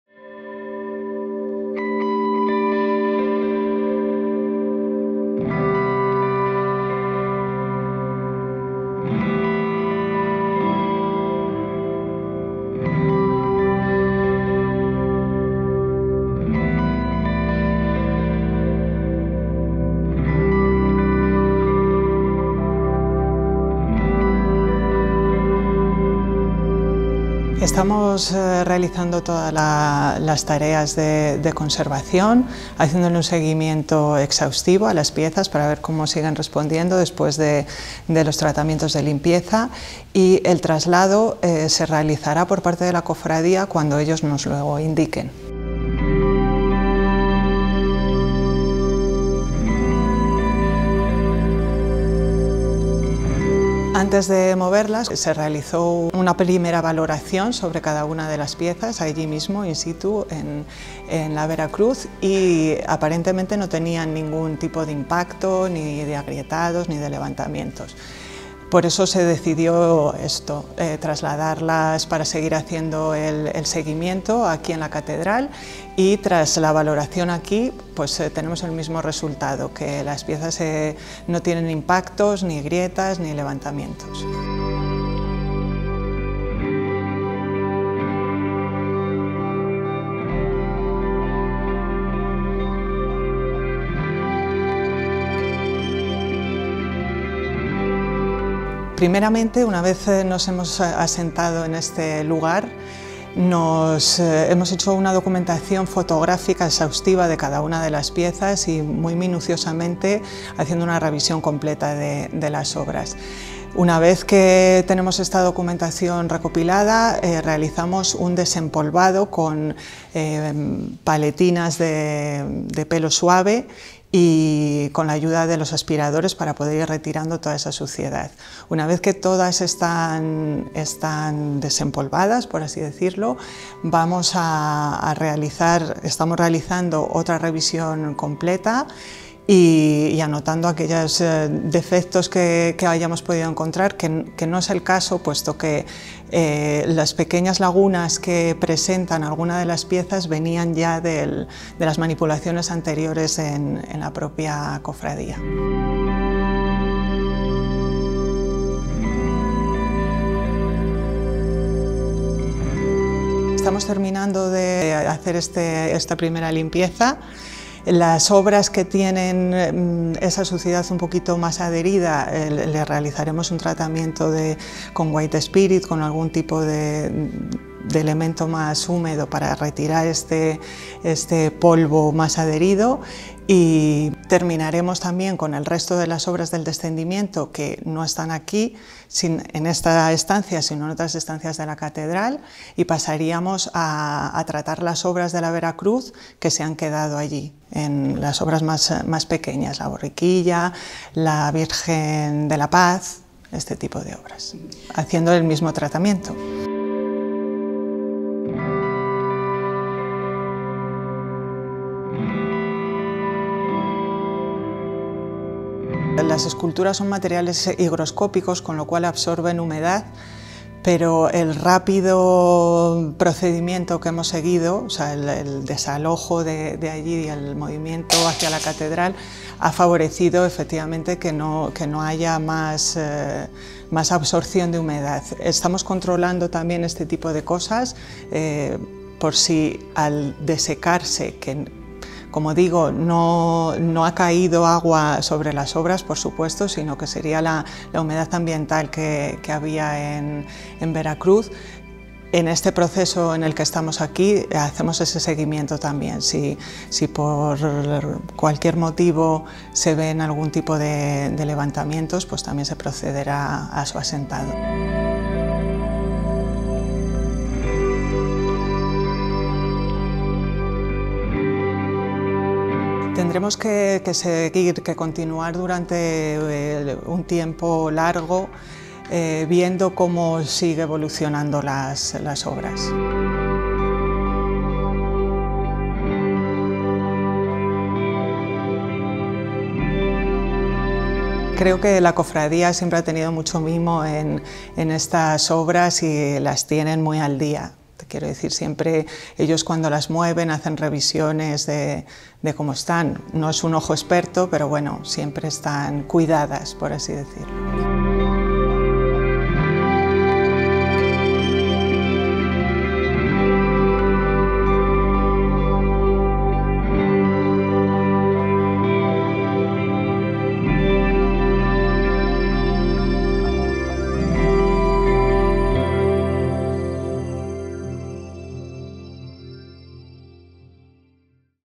La Catedral se convierte en taller de campaña para las imágenes trasladadas desde la Vera Cruz
3-7-2024 - La Santa Iglesia Metropolitana Catedral de Valladolid se ha convertido en un improvisado taller, en el que los técnicos de la Fundación Las Edades del Hombre trabajan como si se tratara de un hospital de campaña, para las imágenes que fueron trasladadas desde la Iglesia Penitencial de la Santa Vera Cruz tras el reciente colapso de su cúpula.

La estampa es inédita. En una misma sala, cedida por la Archidiócesis de Valladolid, nueve obras talladas en madera policromada por Gregorio Fernández, Andrés de Solanes y Pedro León Sedano. Basta un solo movimiento de cabeza hacia la derecha para pasar de mirar cara a cara a ‘El Señor Atado a la Columna’ a cruzar la mirada con el ‘Ecce Homo’, que parece fijar sus ojos en los del espectador con la misma intensidad que la corona de espinas se clava en el comúnmente conocido como “Cristo de la Caña”; mientras ‘Nuestra Madre la Virgen de los Dolores de la Vera Cruz’, por haber tenido que descender urgentemente del Altar Mayor al suelo del taller, parece ahora querer abrazar ya no a su hijo en ‘El Descendimiento’ —paso del que formó parte en origen— sino al mismo espectador que asiste a las labores de limpieza de todas estas tallas.